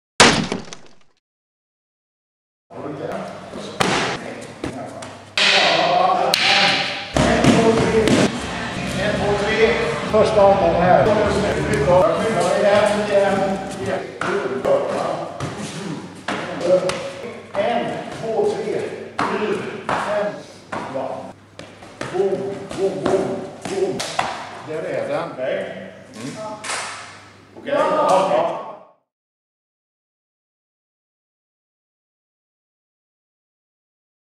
Kali Sikaran: Sikaran med mera
Lite videoklipp från en träning med fokus på Sikaran (filippinsk kickboxning), men även lite käppar, knivförsvar och Kadena de Mano (filippinskt vapenlöst självförsvar på kort distans).

Some videoclips from a training with a focus on Sikaran ( Filipino kickboxing), but also some sticks, knife defense and Kadena de Mano ( Filipino self-defense in a short distance without weapon).